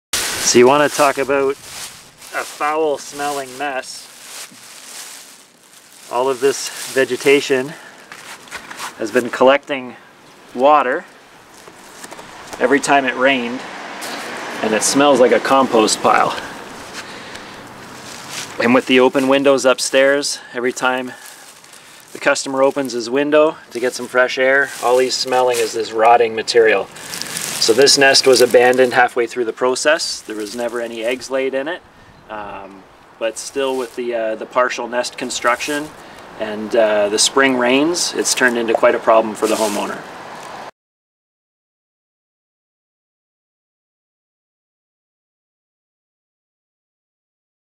Messy Bird Nest Pulled from Wall Vent
Nobody wants this sitting in their wall vent! Our technicians pulled out this bird's nest that had been collecting moisture and cutting off circulation from a homeowners wall vent. When birds choose your home as their nesting site, there can be fire hazards created, and bacteria left behind. If you suspect you have birds living inside a vent in your home, visit our website below to begin your removal process to remove the nasty nest before the mess spreads!